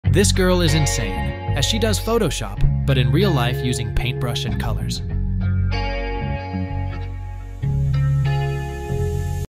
Real Life Photoshop Skill ❤️
🎥 @millybampainti On IG

This girl is insane, as she does photoshop but in real life using paintbrush and colors